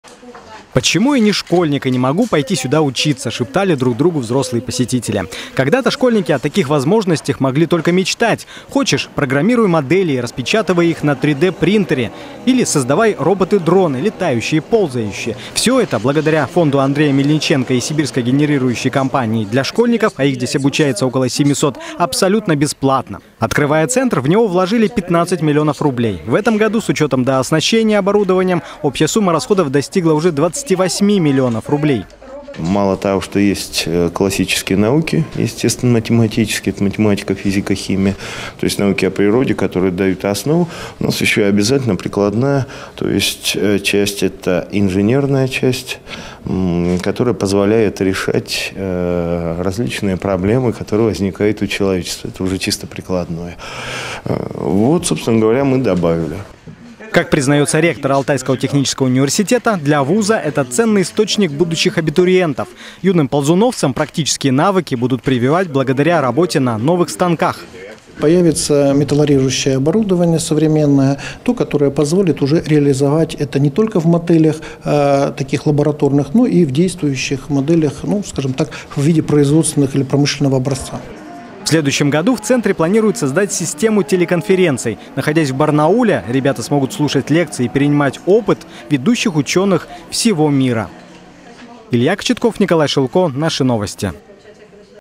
В центре «Наследники Ползунова» открыли две новые лаборатории – программирования и робототехники
Воспитанники и преподаватели презентовали свои проекты и провели открытые занятия.